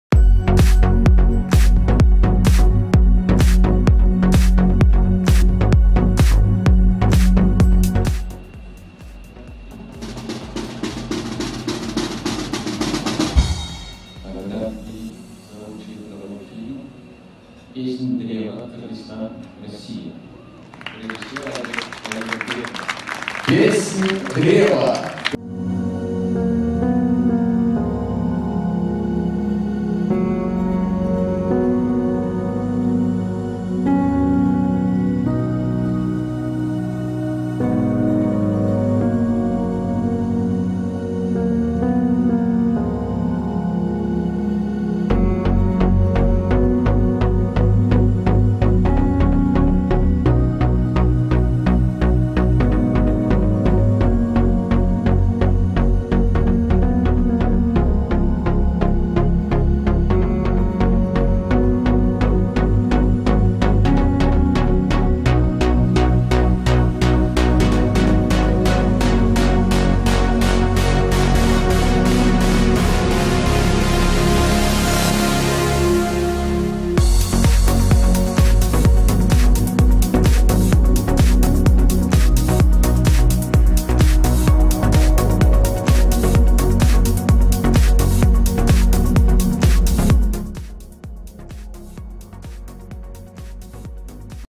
5 ГЛАВНЫХ СОБЫТИЙ ФЕСТИВАЛЯ МУСУЛЬМАНСКОГО КИНО
В Казани подвели итоги кинофорума. Главный приз едет в Кыргызстан